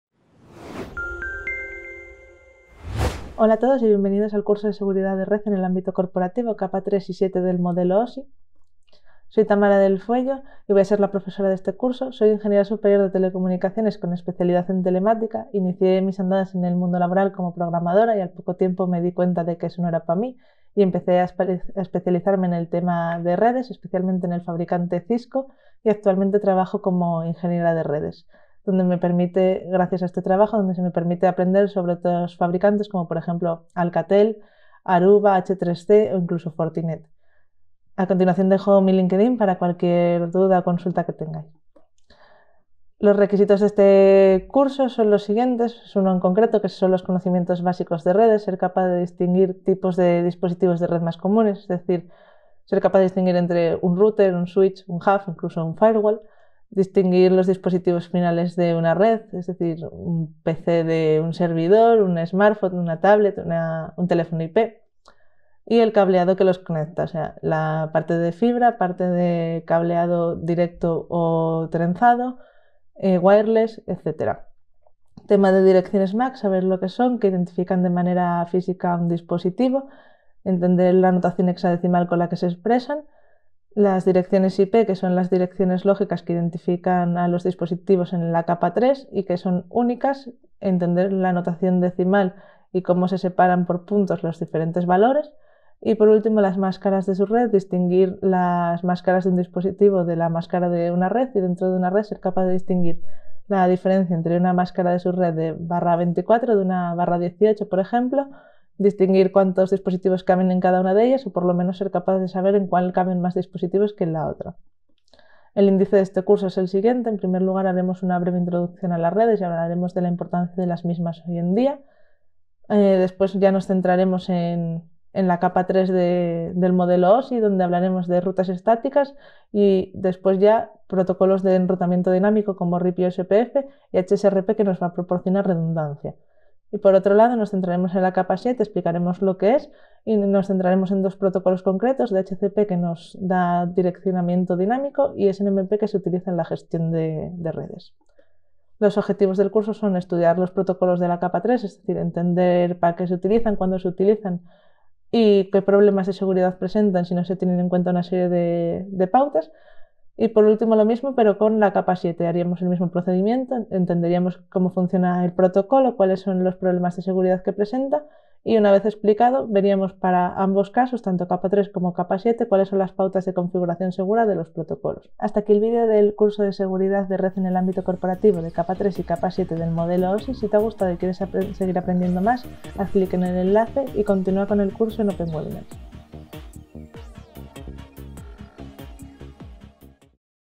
Curso de Seguridad de red en el ámbito corporativo: Capas 3 y 7 del modelo OSI
En este curso vamos a ver los errores más comunes al realizar la configuración de algunos protocolos de red más en el ámbito corporativo y cómo corregirlos, concretamente nos centraremos en los relacionados con las capas 3 y 7 del modelo OSI.

- Realizar estudio de protocolos de capa 3: RIP, OSPF, HSRP.
- Comprender los protocolos de capa 7: DHCP, DNS, SNMP.
- Aprender una serie de pautas para una configuración segura de estos protocolos.

Comienza este Curso de Seguridad de red en el ámbito corporativo: Capas 3 y 7 del modelo OSI